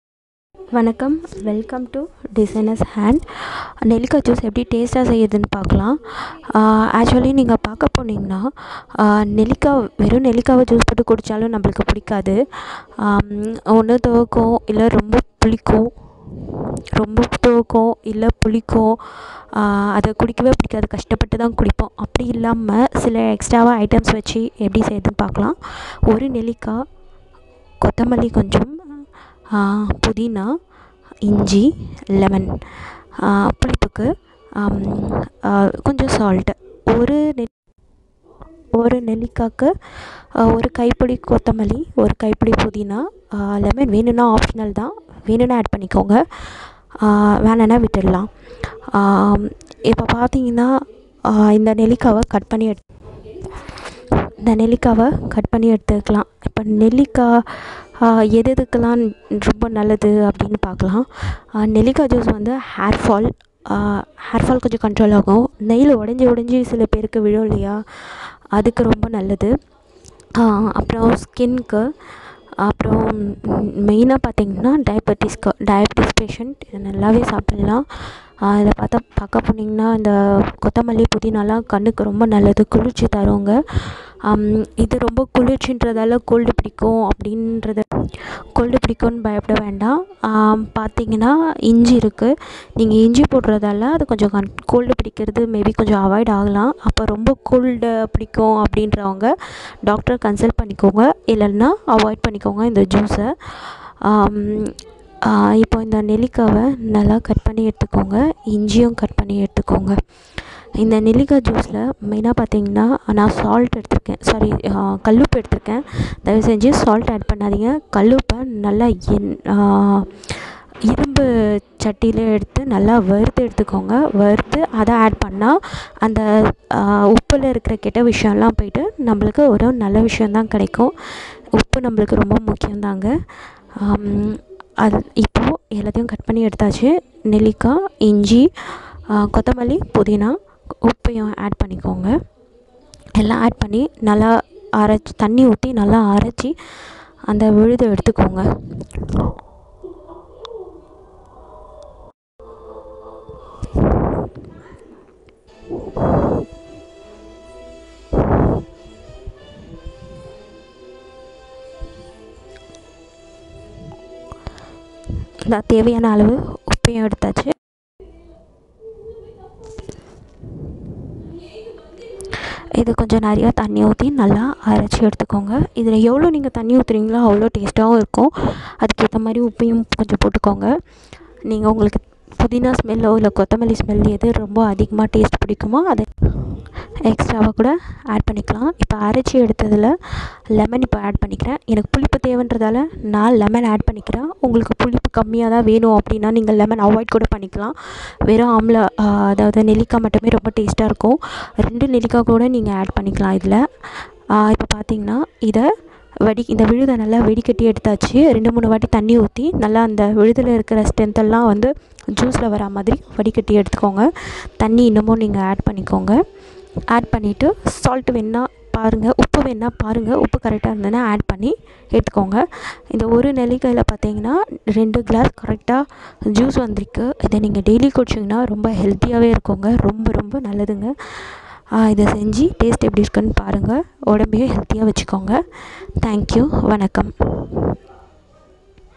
Amla juice receipe / health drink / tasty amla juice / nellikai juice / designers hand
Amla juice receipe / health drink / tasty amla juice / nellikai juice / designers hand / in Tamil / nutritional drink
ingredient:
Amla-2
corriander
mint leaves
lemon - 1/2
salt
water.
Ginger
Amla juice preparation
Cut amla, Ginger into pieces
Add amla, Ginger, corriyander, Curry leaves, water in mixy and grind it to make paste
Filter it 2 times using water
Now add lemon, salt, required water  in it, now the tasty healthy nutritional amla juice is ready to serve

Benefits:
control hairfall
good for skin and nails
Contain vit-C
Eating amla observes vit-D from sunlight, which is good for bone